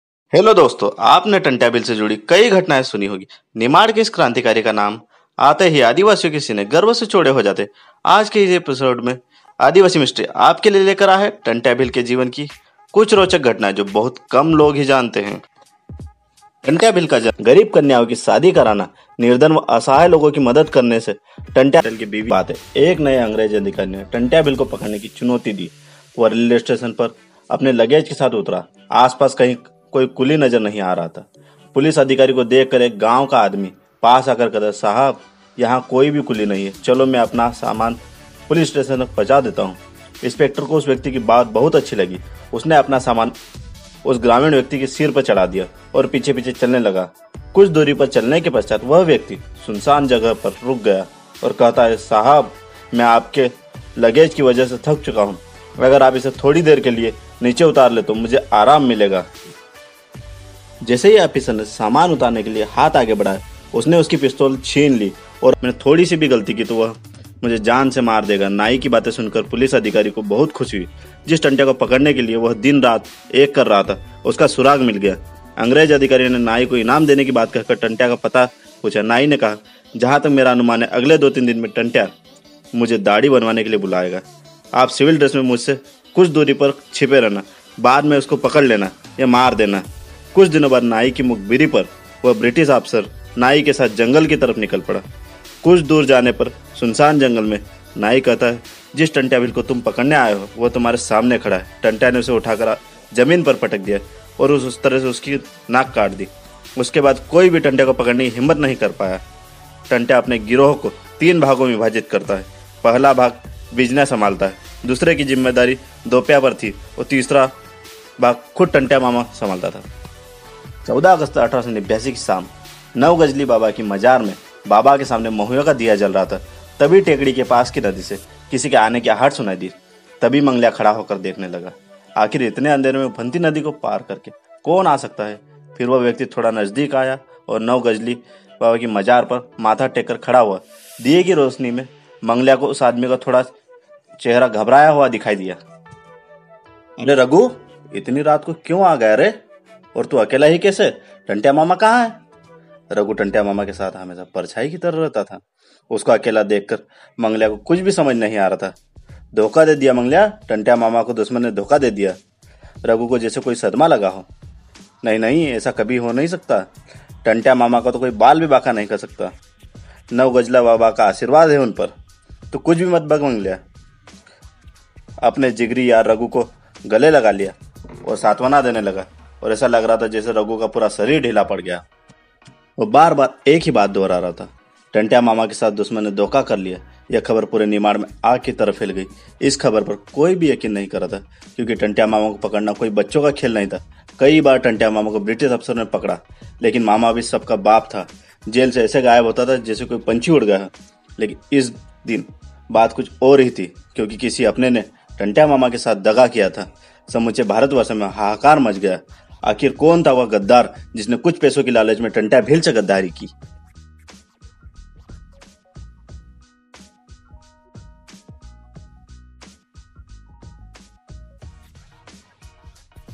Tantya Mama की सम्पूर्ण आत्मकथा | Tantya Bhil full Story| Tantya Bhil Movie Part-1
Tantya Bhil full story । Tantya Bhil movie। Tantya Bhil। Tantya Mama TantyaMama Tantya Bhil Aadiwasi Mystery ke is episode me Bharat k veer Shahid krantikari Tantya Bhil ke jivan se judi kuch ghatnao ko kahani ka rupantaran diya gya h, Tantya mama bharat k pratham swatantrata sangram k veer krantikariyo me se ek hai. Aadiwasi Tantya mama aaj bhi bhagwan ki tarah puje jate h. Tantya Bhil sadev bhartiyo k liye prerana ka ek strot rahenge. Yugo Yugo tak Bharat k veer Tantya mama ko yaad kiya jayega. Team Aadiwasi Mystery Tantya Bhil se bahaut prabhavit hui, Unko ek choti si shradhanjali arpan krne k liye Aadiwasi Mystery k dwara ek prayas kiya gaya h. Aadiwasi yodha, Aadiwasiyo k bhagwan, Kranti Surya, Nimar k Raja , Bhagwan Tantya Bhil sada hamare dil me jivit rahenge. Jai Tantya Mama, Jai Johar. " # Aadiwasi Mystery #"